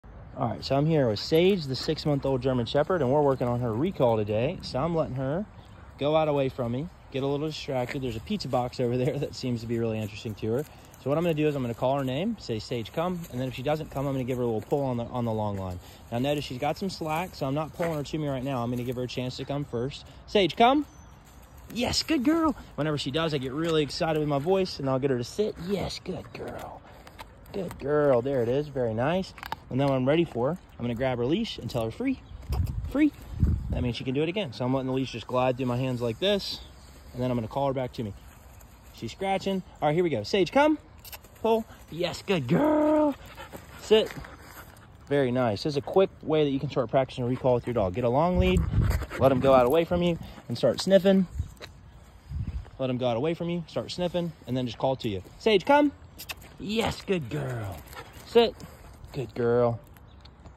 Quick Recall Exercise to Try W/ Your 🐶
Recall! One of the most important things you can teach your 🐶. This is a super basic way to practice. Grab a long leash, some treats and take your pup out.
Let them walk out ahead, give a clear and firm “come” or “here” command. Wait a second and if they don’t come, give them a tug to you on the long leash. Once they do start moving towards you, get really excited and mark them moving towards you with a “YES” or “GOOD GIRL” and make it fun to come to you!
Once they do come to you, give them a treat to reward them and lots of lovin’!
Bonus: you can get them to sit and wait on YOU to give them their password or release command to go back out to sniff.
Try it and let me know how it works for you 🐕